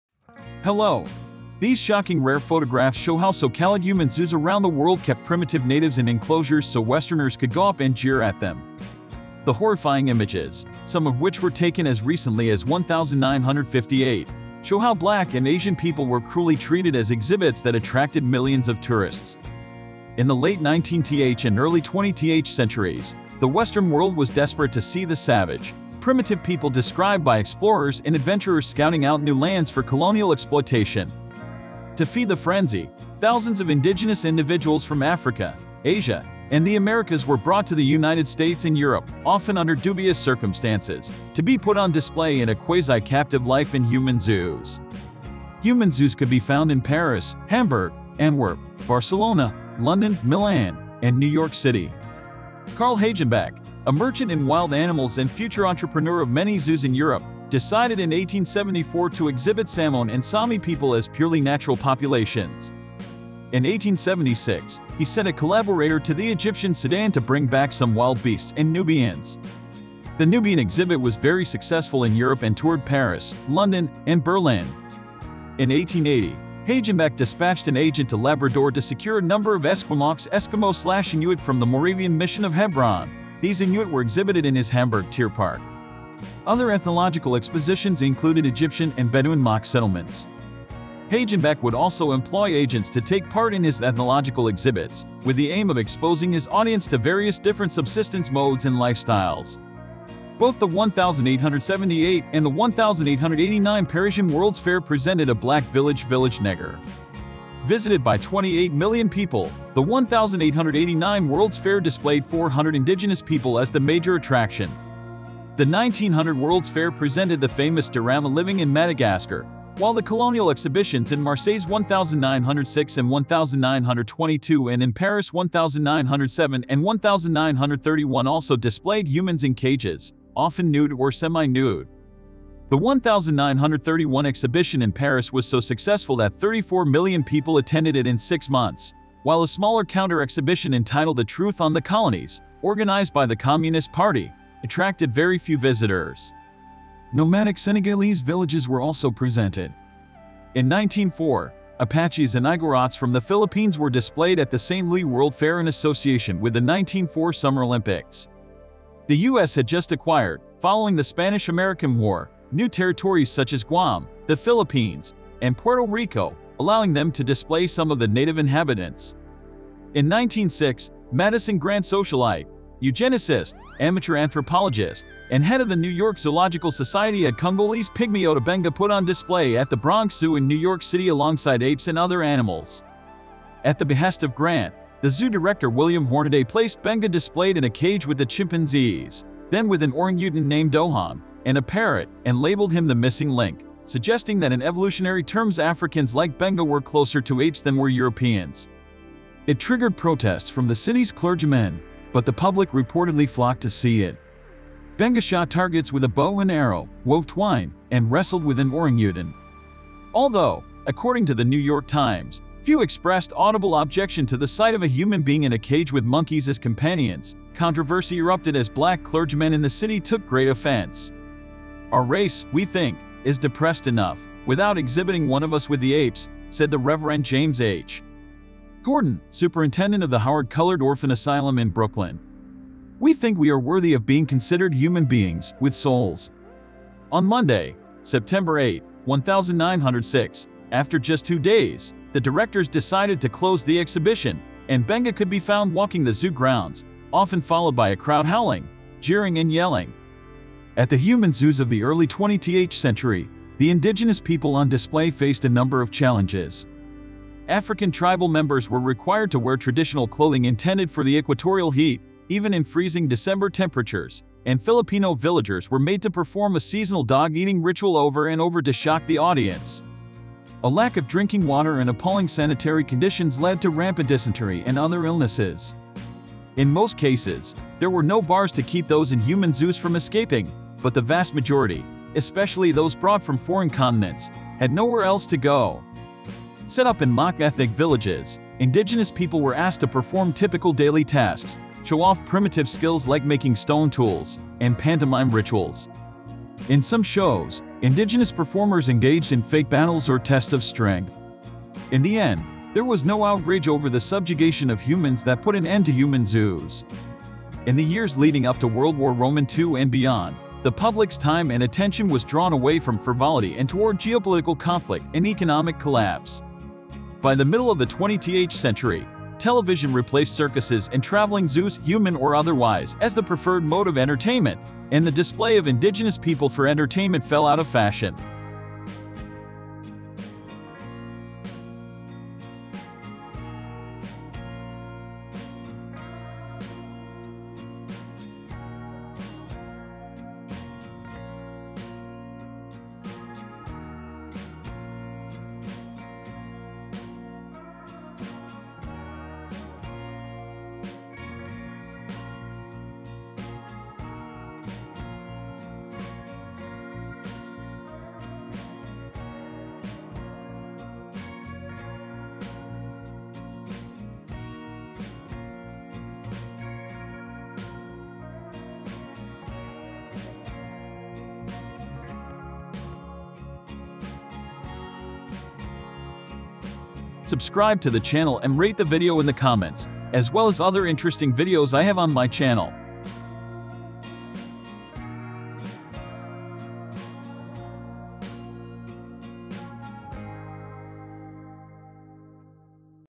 Human Zoos: Revealing the Western World's Shameful Secret, 1900-1958
In this thought-provoking video, we delve into a dark and often overlooked chapter of history – the existence of human zoos in the Western world from 1900 to 1958. Uncover the disturbing truth behind these exhibitions that treated people from different ethnicities as objects to be displayed for public entertainment. Through a combination of archival footage, historical photographs, and expert analysis, we shed light on the shocking reality faced by individuals who were subjected to such dehumanizing exhibits. Explore the motivations behind the creation of human zoos, the global impact of these spectacles, and the subsequent efforts to bring an end to this shameful practice. This video aims to raise awareness, promote understanding, and encourage dialogue about the ethical implications of such displays, ensuring that this dark chapter of history is never forgotten. Join us as we uncover the uncomfortable truths surrounding human zoos and strive for a more inclusive and empathetic world.